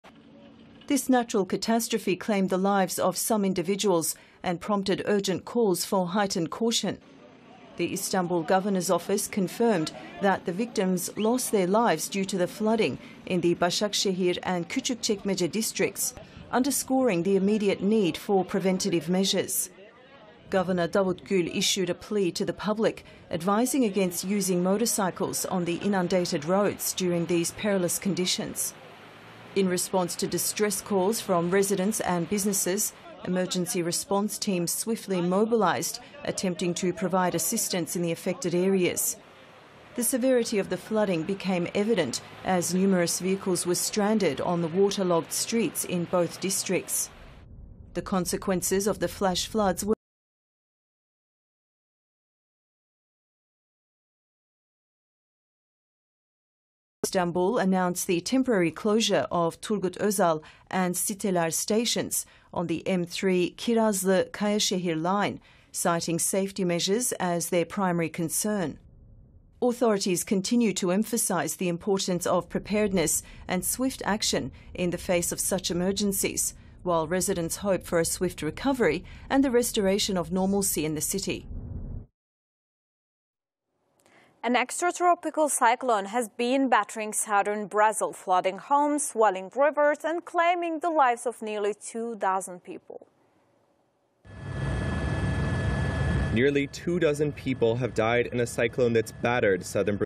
Flash floods wreak havoc in Istanbul's Arnavutköy and Başakşehir districts
Istanbul, Türkiye's vibrant metropolis, experienced a devastating turn of events on Tuesday night as heavy downpours in the northern part of the city led to flash floods that wreaked havoc in the Arnavutköy and Başakşehir districts.